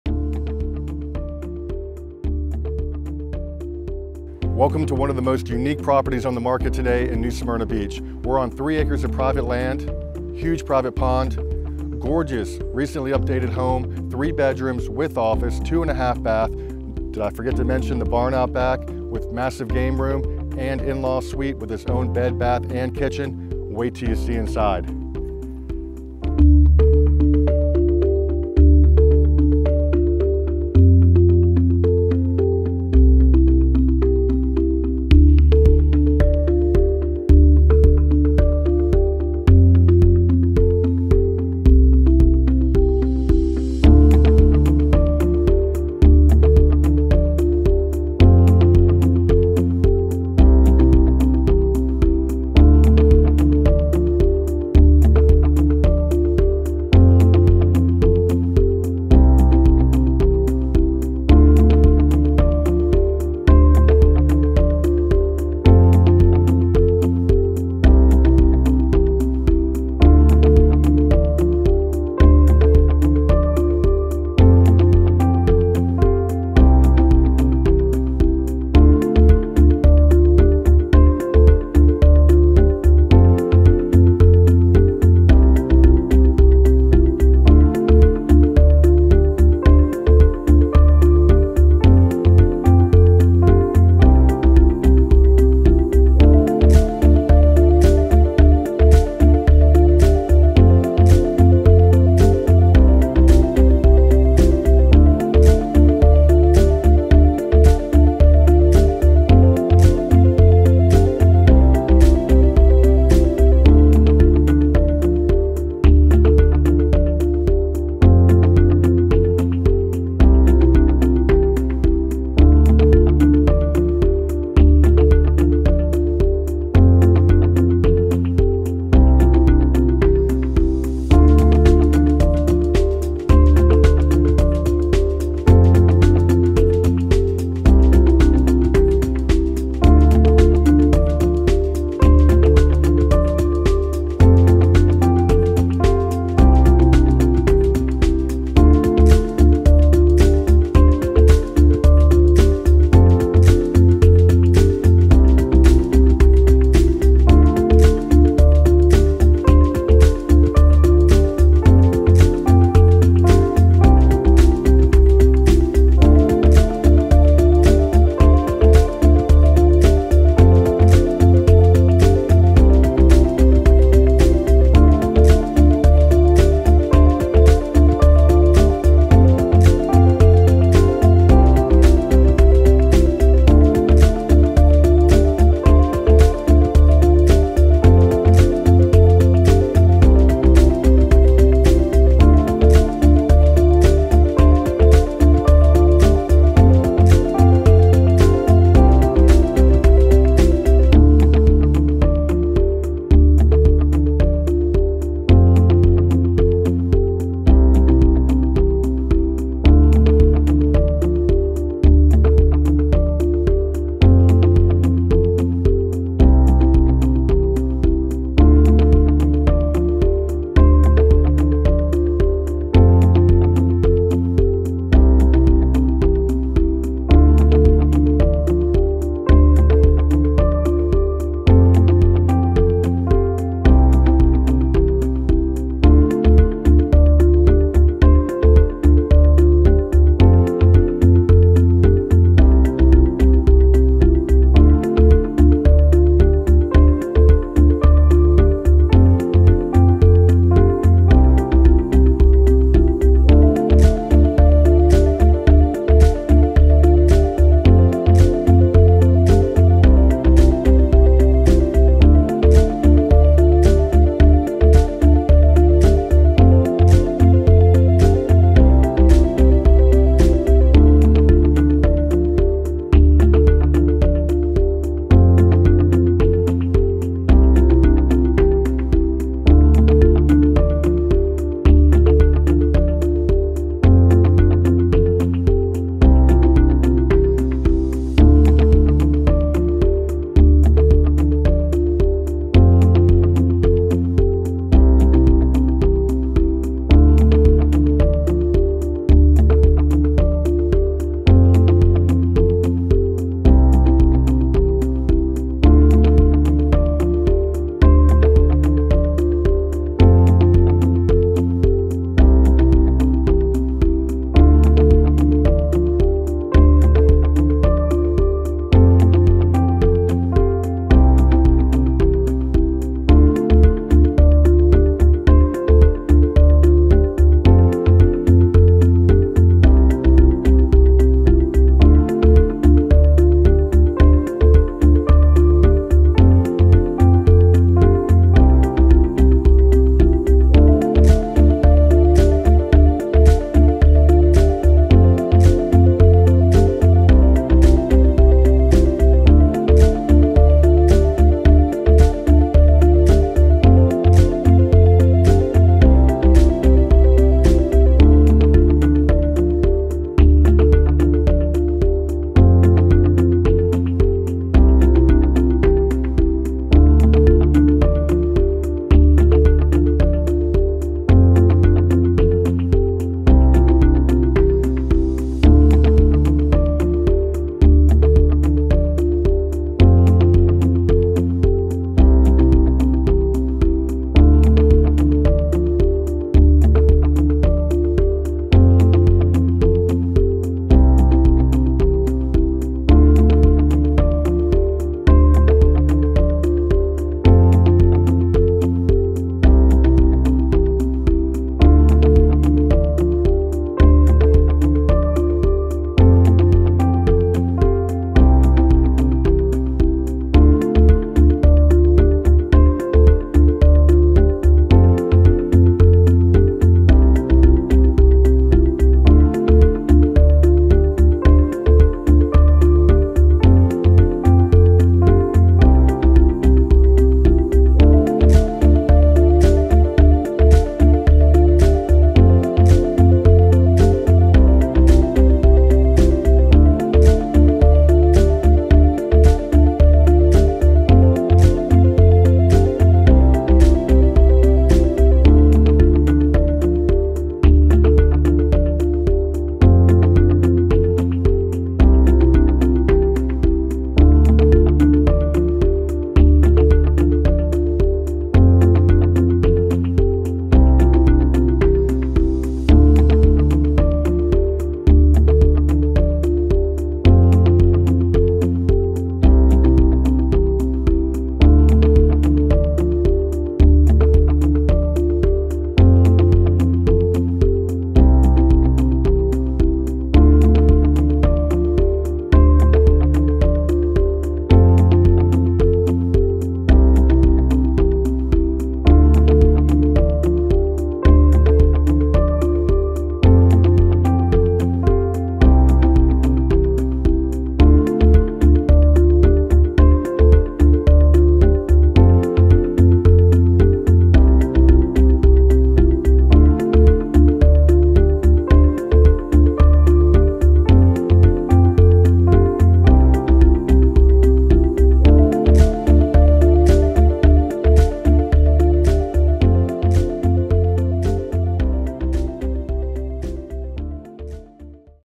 2646 Turnbull Bay Rd, New Smyrna Beach, FL 32168
The most unique listing in new Smyrna Beach on the market today!  Enter this private fully fenced 3 acre estate through the gated entrance and take in the tree lined driveway with canopy of majestic Oaks. Multiple structures, all situated around a large private lake with a fountain.
The main home consists of three bedrooms, a dedicated office and 2 1/2 bathrooms.  Enter through the beautiful bevel glass double entry front doors into the large and welcoming foyer. You immediately noticed the new porcelain tile flooring consistent throughout the home.  Open concept chefs kitchen with gas range, shaker cabinetry, quartzite countertops, and large Butler’s pantry.  Easily accessible from the kitchen is the formal dining room with lovely architectural features such as crown molding, plantation shutters, and built-in shelving.  The family area is perfect for entertaining and loaded with amenities, such as a dry bar with leather finished countertop, gas fireplace, shiplap wall, details, and built-in surround speakers.  The owner suite offers an incredible amount of closet space with three walk-in closets and a newly renovated bathroom loaded with custom cabinetry and a massive spa-like shower. There is a private screened porch directly off of the owner's bedroom with a hot tub. Take in the relaxing views of the private lake and expansive property.  The split bedroom concept offers privacy and the newly renovated Jack and JILL bathroom connecting the two secondary bedrooms is perfect for guests. Work from home or use as a possible  fourth bedroom from the dedicated office space. The outdoors flow seamlessly into the interior via a large screened lanai where the house flooring continues throughout along with ceiling fans to make the afternoons even more enjoyable.  Other features include central vacuum, updated lighting and fans throughout as well as a newly renovated powder room. A small stream runs through the center of the property with a footbridge, and two small outbuildings for additional storage. The large detached  barn offers a finished game room with half bath and built-in bar fantastic for entertaining and family gathering downstairs. Upstairs  features an in -law apartment with bedroom, bathroom, living room and kitchen with private entry.  Tons of opportunity for expansion, RV parking, boat parking, trailer, and multiple vehicle storage.  Seize the opportunity as Properties such as these rarely become available.